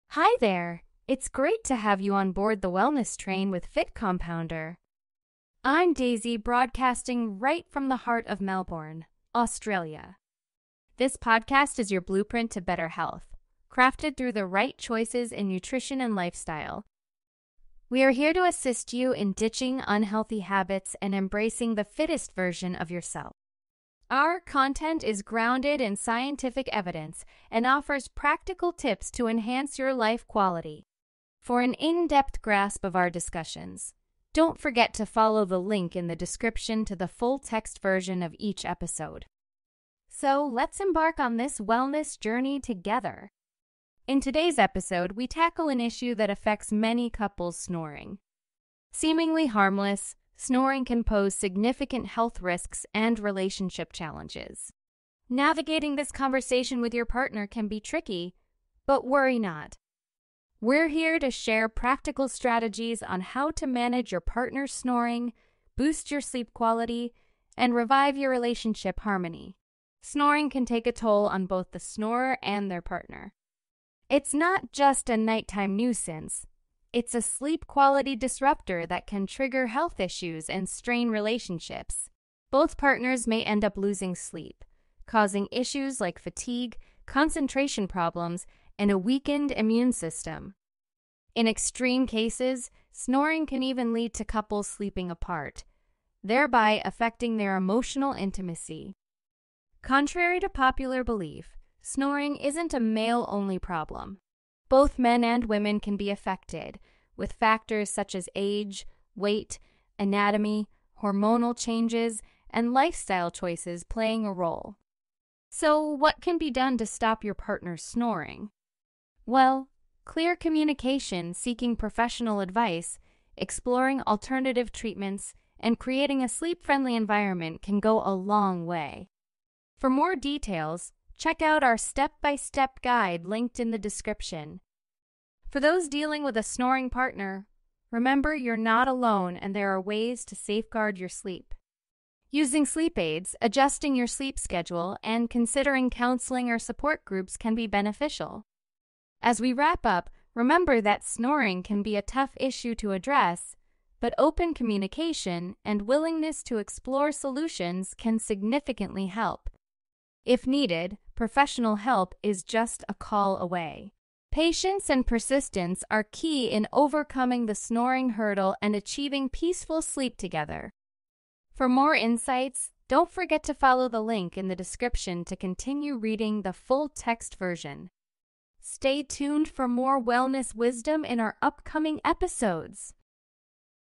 How to Stop Partner Snoring: Revive Your Relationship
These easy throat exercises defeated the most stubborn snoring and sleep-apnea the very first night